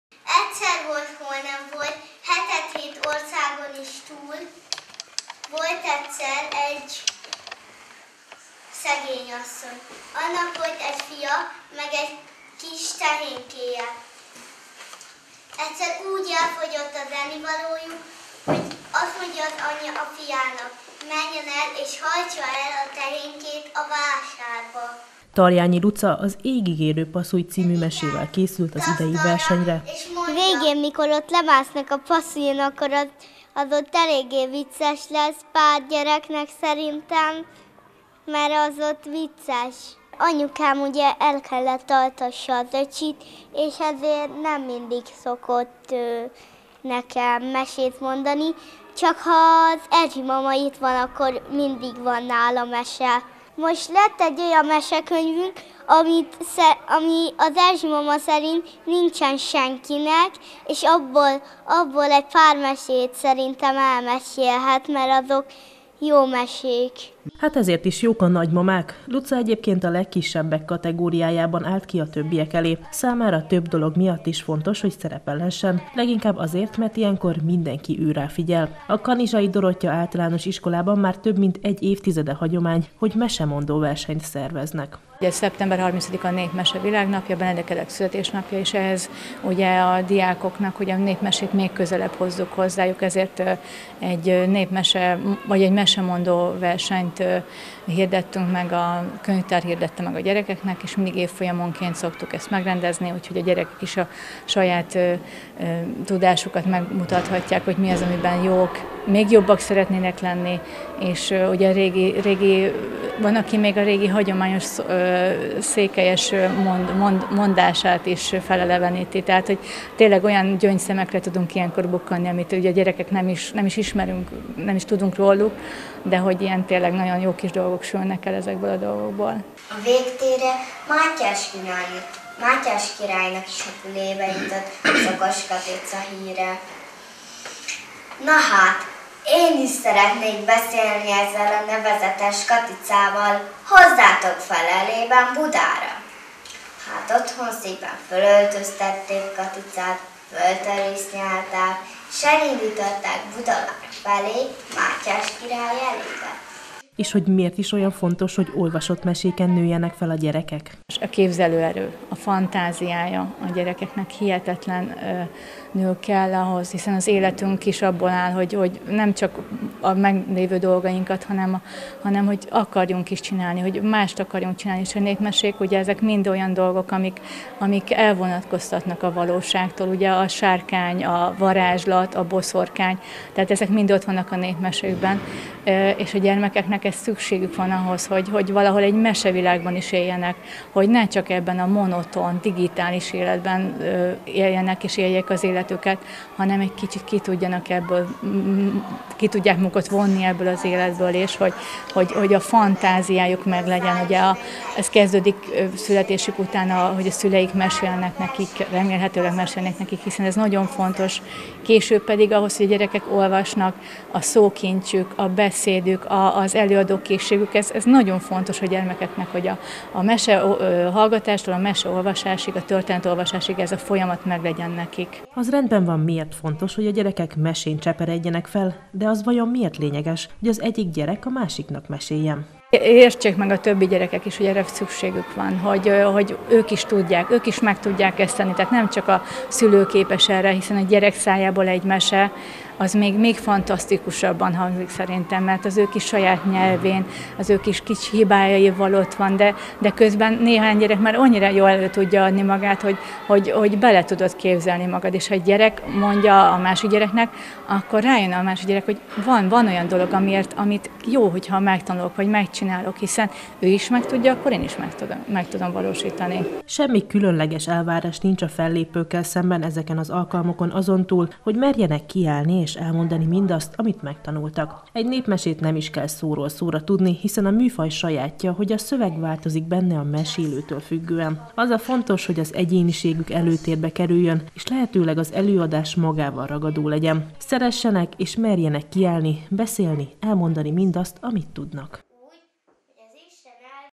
Mesemondó verseny - Bátaszék
A Kanizsai Dorottya Általános Iskolában minden esztendőben rendeznek mesemondó versenyt Benedek Elek születésnapja alkalmából. A kiírás szerint népmesékkel kell készülniük a résztvevőknek, akik koruk szerint több kategóriában mutathatják meg felkészültségüket.